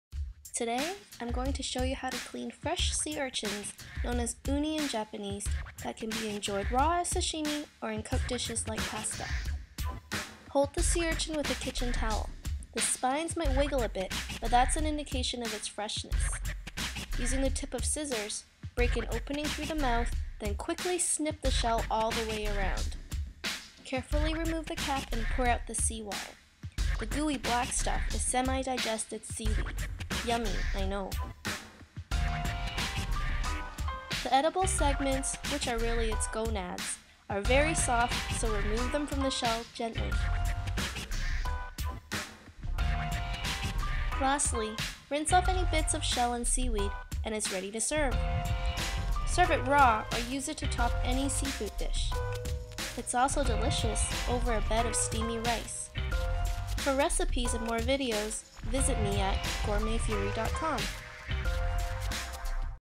Cleaning and Serving Sea Urchins (Uni)
Melody of GourmetFury.com shows you how to prepare live sea urchin (uni) for sashimi, pasta, or other yummy goodness.